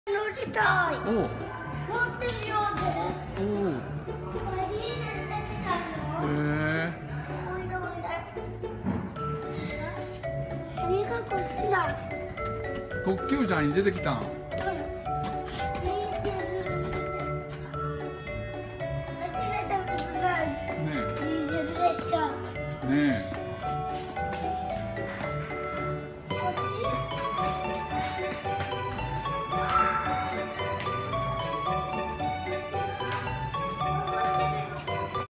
愛媛県西条市 鉄道文化博物館 烈車戦隊トッキュウジャーに出てくるというディーゼル列車 DE101
愛媛県西条市の四国鉄道文化博物館の展示されていた列車です。
特別展示列車「ホビートレイン」のほかにも列車がてんじされていました。
子どもがディーゼル列車を知っていたのでビックリしました。
トッキュウジャーファンの方、間違えてたらすいません。